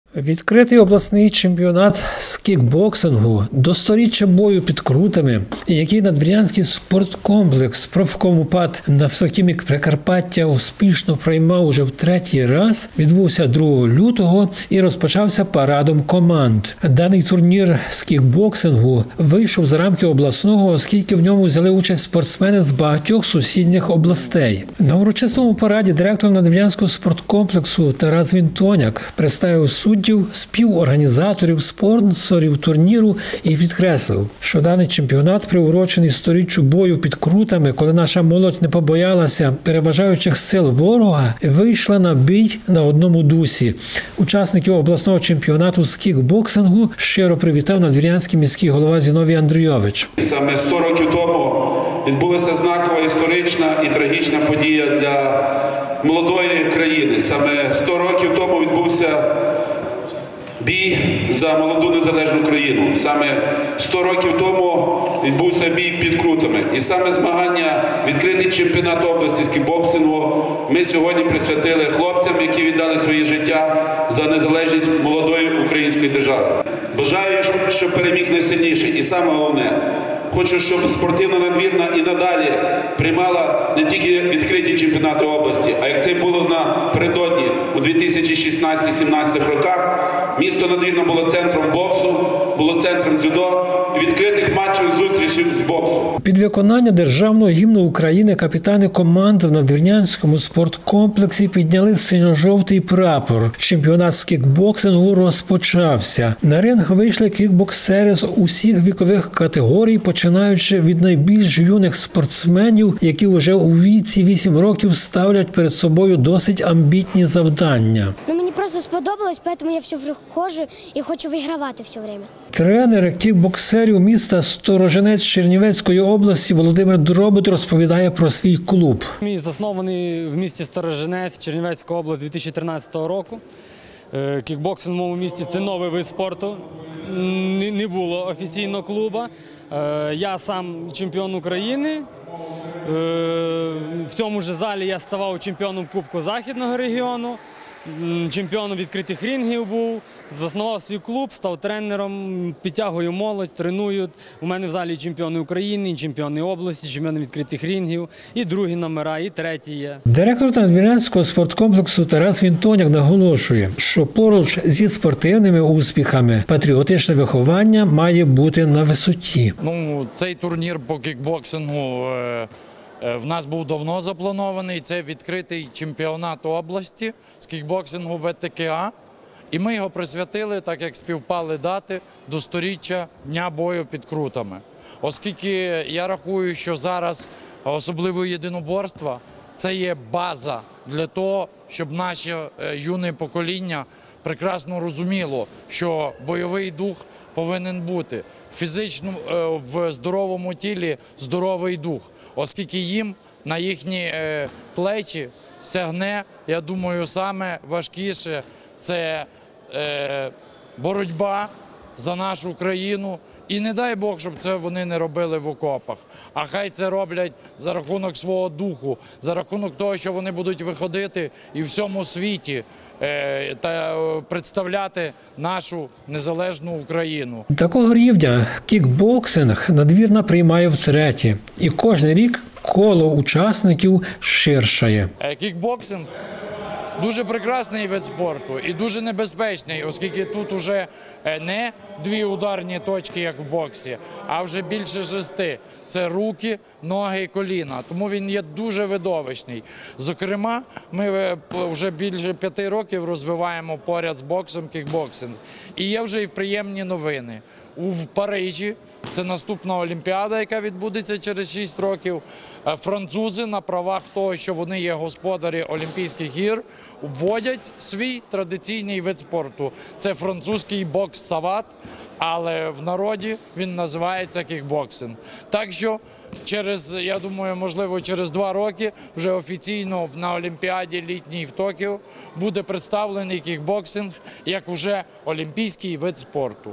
Обласний чемпіонат з кікбоксингу
3-го лютого в Надвірнянському спорткомплексі профкому ПАТ «Нафтохімік Прикарпаття» пройшов відкритий обласний чемпіонат з кікбоксингу, цього року приурочений до 100-річчя бою під Крутами. Такого рівня відкрита обласна першість з кікбоксингу у Надвірній відбувається втретє.